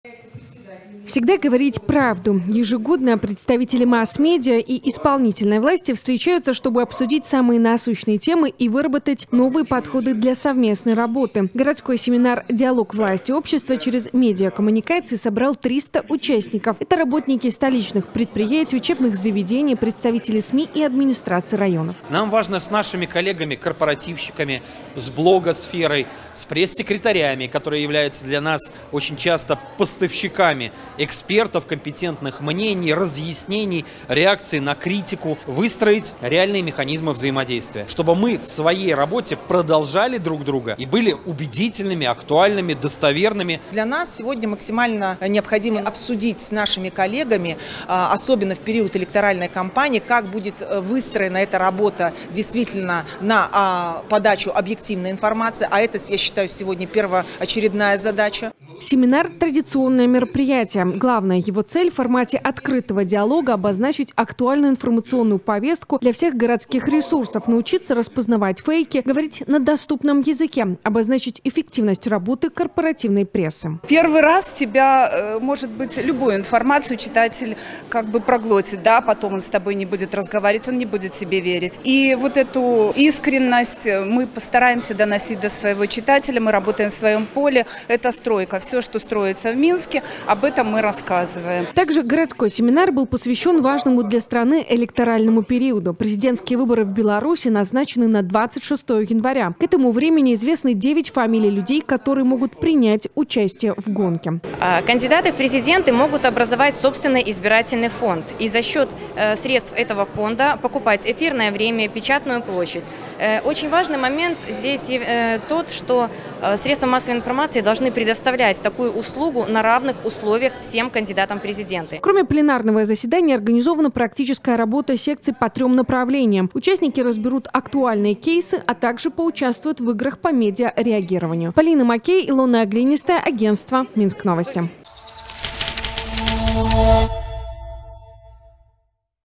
Диалог власти и общества через медиакоммуникации || В Минске состоялся городской семинар
Всегда говорить правду. Ежегодно представители масс-медиа и исполнительной власти встречаются, чтобы обсудить самые насущные темы и выработать новые подходы для совместной работы. Городской семинар «Диалог власти и общества через медиакоммуникации» собрал 300 участников – это работники столичных предприятий, учебных заведений, представители СМИ, администраций районов и общественных объединений.

Полина Маккей, Илона Глинистая, агентство "Минск-новости"